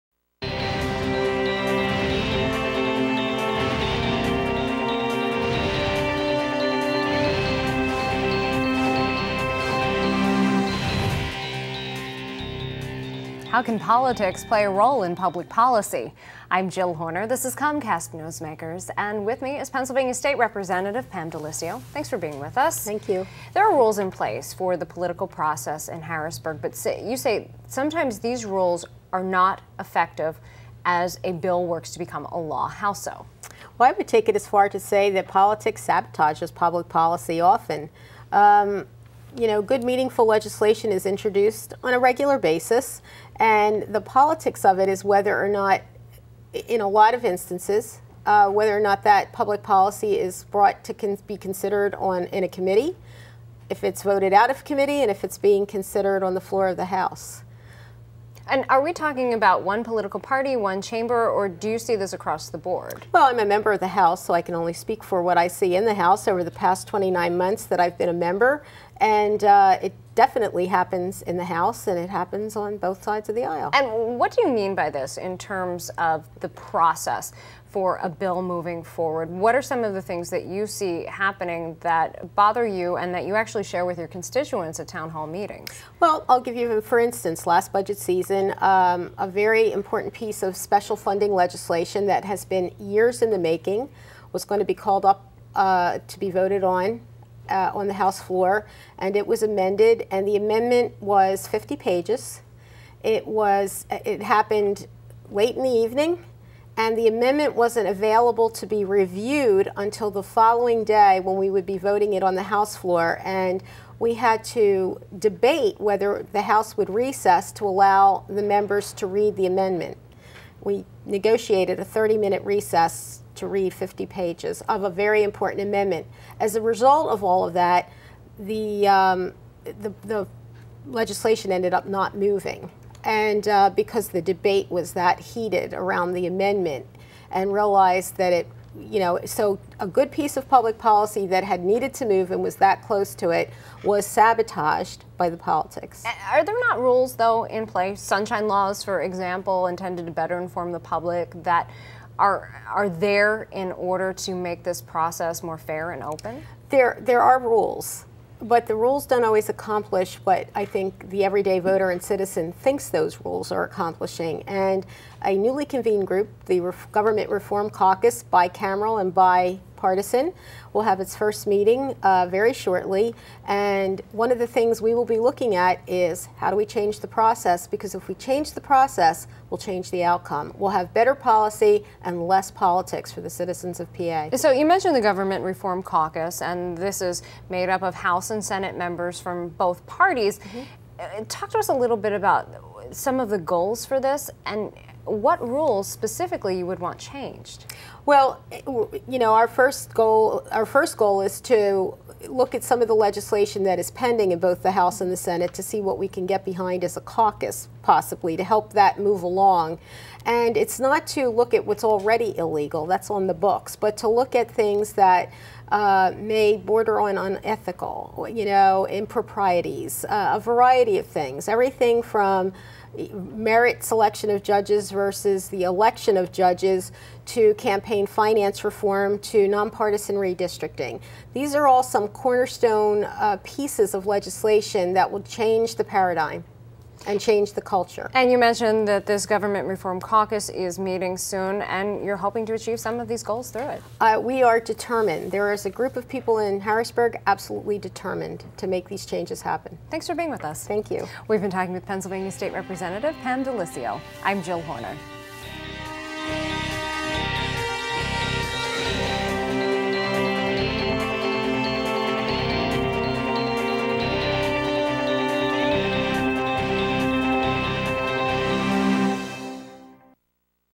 DeLissio Criticizes Process
As a member of the Government Reform Caucus, Pa. state Rep. Pam DeLissio discusses how politics can derail public policy.